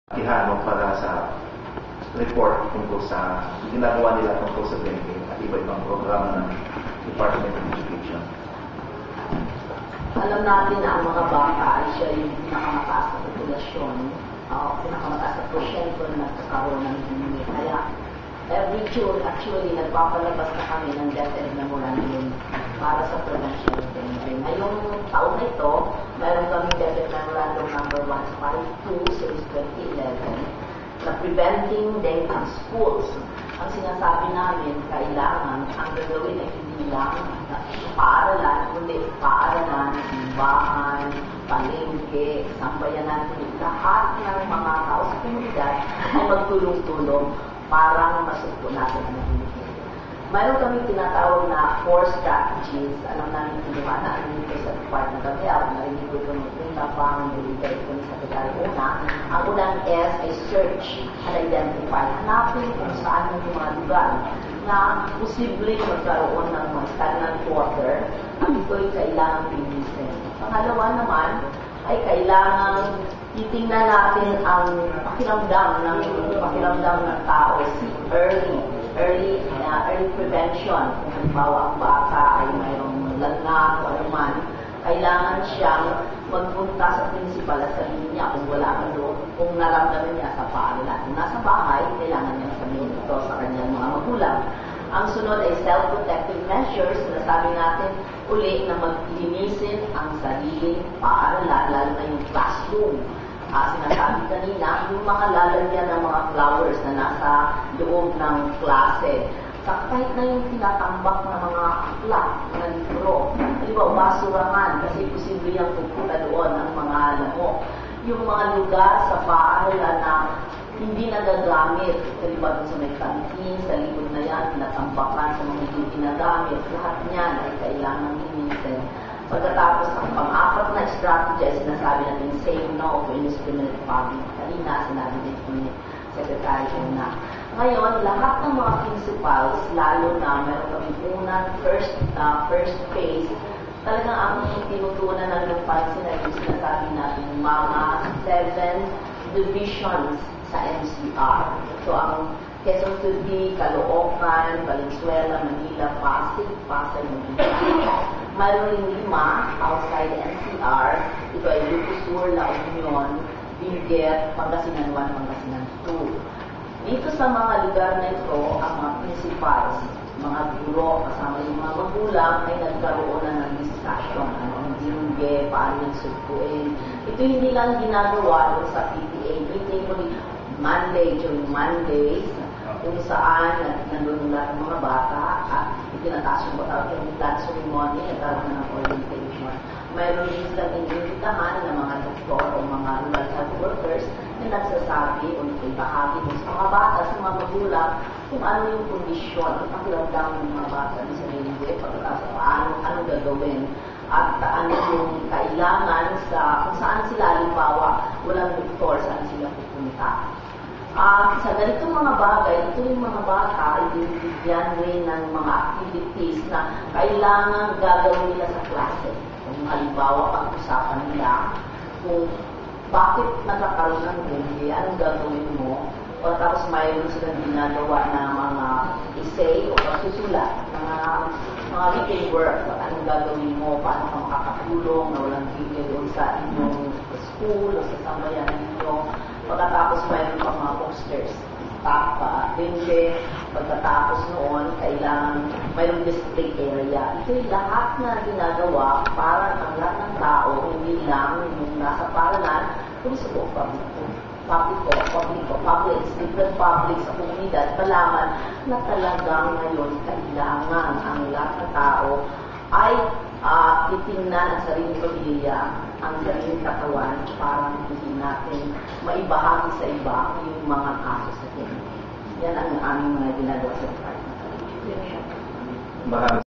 Kampanya kontra Dengue, pinalakas pa ng DepEd
Pinag-iibayo ng Department of Education ang kampanya kontra dengue sa mga paaralan.

Sa pang-Biernes na edisyon ng Communications and News Exchange Forum, sinabi ni Education Undersecretary Yolanda Quijano na ginagalugad at sinisira nia ang mga maaring pamugaran at pangitlugan ng mga lamok sa mga kampus.

Para sa karagdagang impormasyon, tingnan ang video link na kung mapapanood ang pagpapaliwanag ni Dr. Jun Araos sa nakaraan DOST-Kusyunan kung saan tampok ang Mosquito OL Trap.

Ang video ay pag-aari ni Lyndon Plantilla ng Philippine Information Agency.